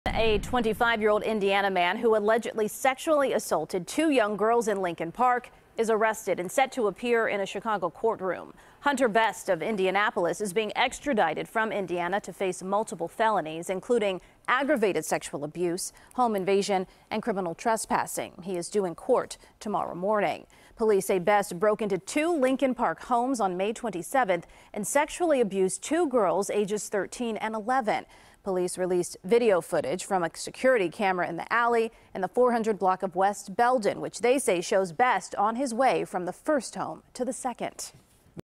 Suspect Arrested In Lincoln Park Home Invasion Sex Assault Case
A 25-year-old Indiana man who allegedly sexually assaulted two young girls in Lincoln Park was arrested and set to appear in a Chicago courtroom.